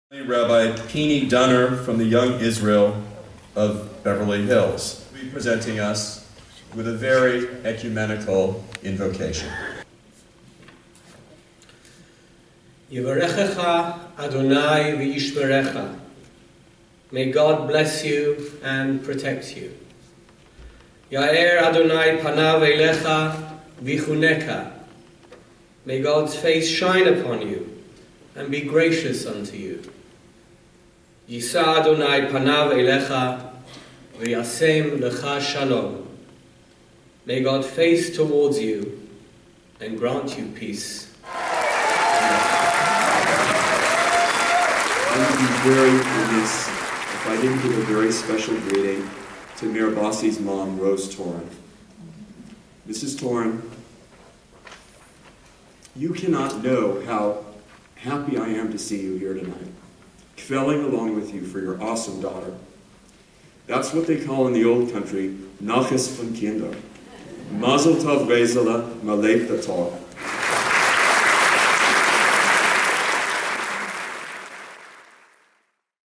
Inauguration of Beverly HIlls Mayor Lili Bosse
Young Israel . Beverly Hills Rabbi Pini Dunner offers the benediction at the Inauguration of Mayor Lili Bosse; with a bissel of yiddish offered  by ouitgoing Mayor John Mirisch